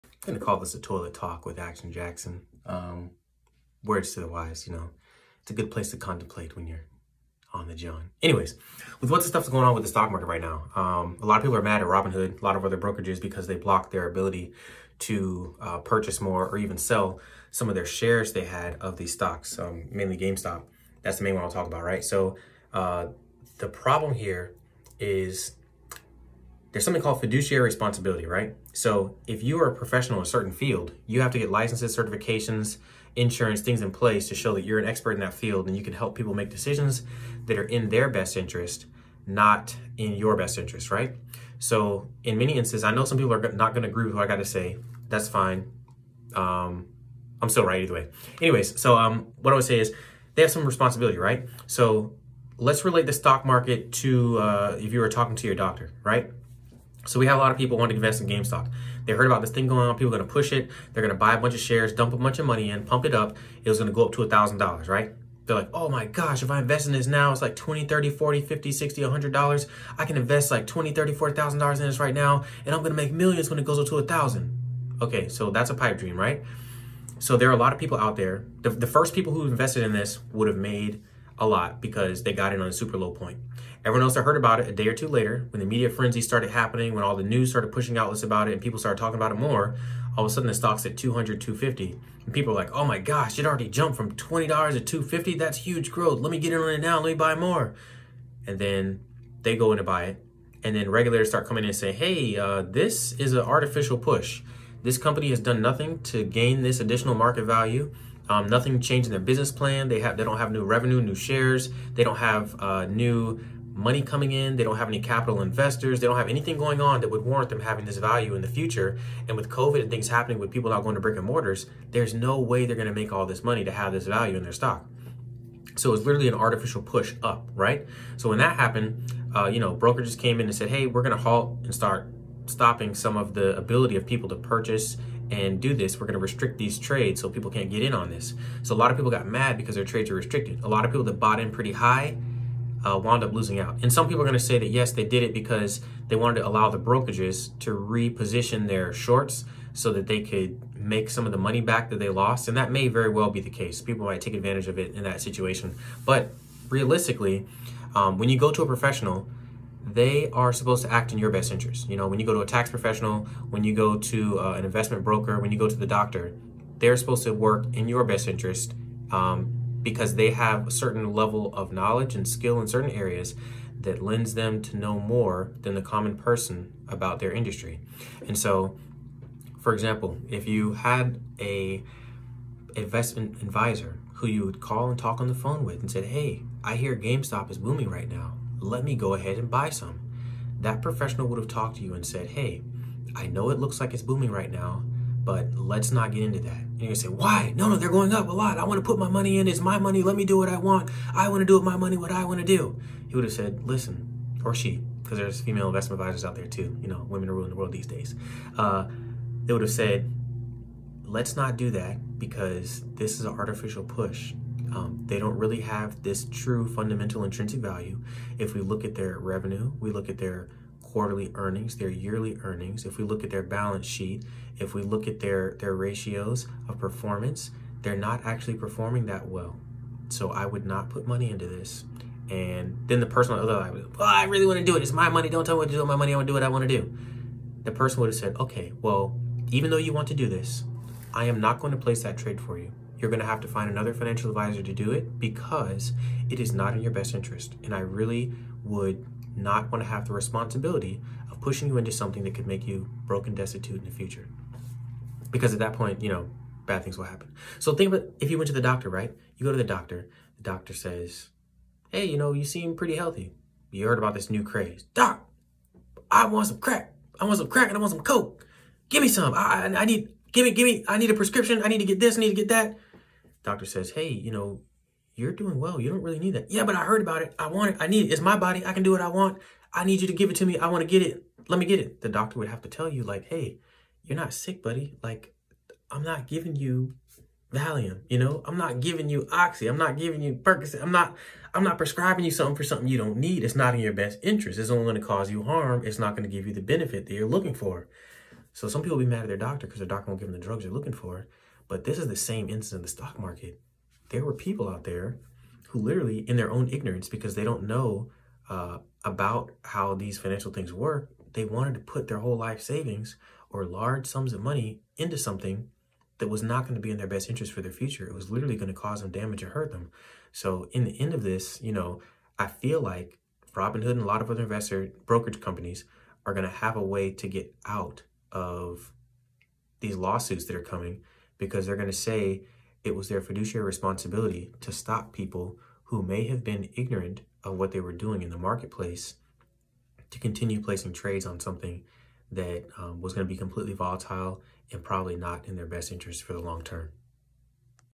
Robinhood and GME aren’t the markets supposed to be free?!
Although I don’t fundamentally agree with what happened in the markets restricting trades and so on, I see what they are doing and what their probable excuse will be when they go to court to face these class action lawsuits.

Here I share a few thoughts on the matter to those who care to listen.